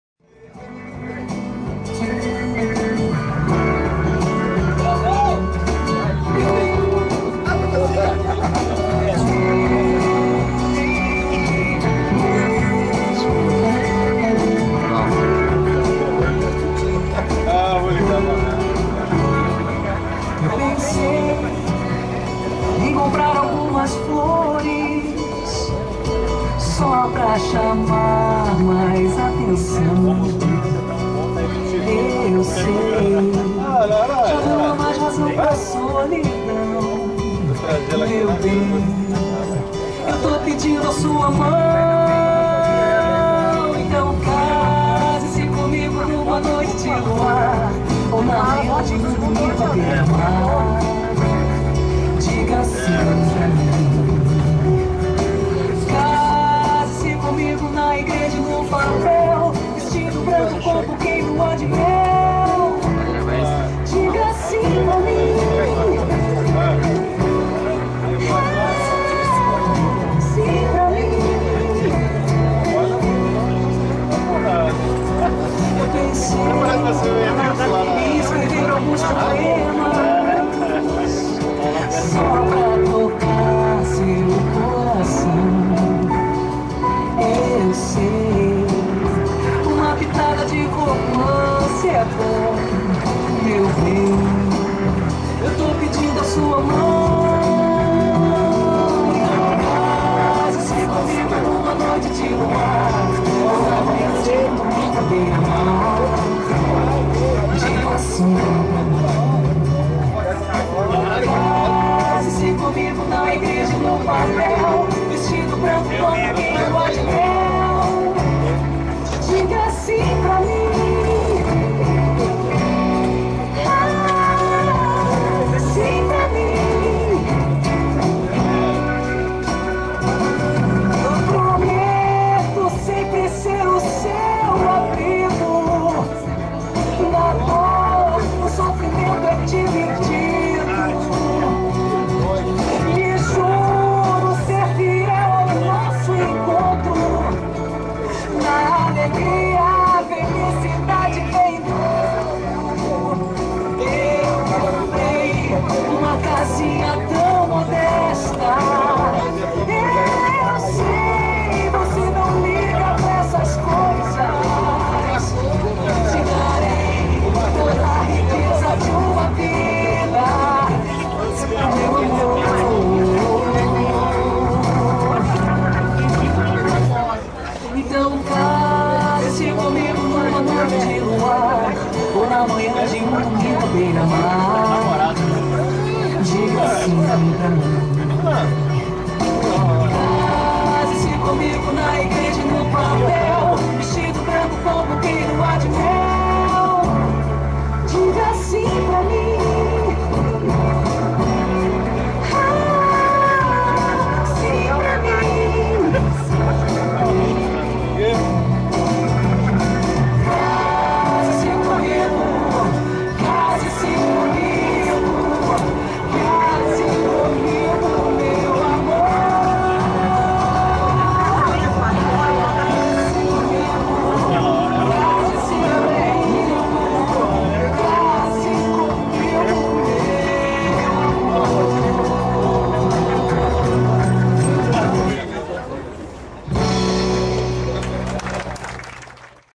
XAXU VIDEO 2
Nossa querida Xaxu em noite de festa no Quiosque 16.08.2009,  no aniversário do nosso queirdo França, nesta bela interpretação de Isabella Taviani!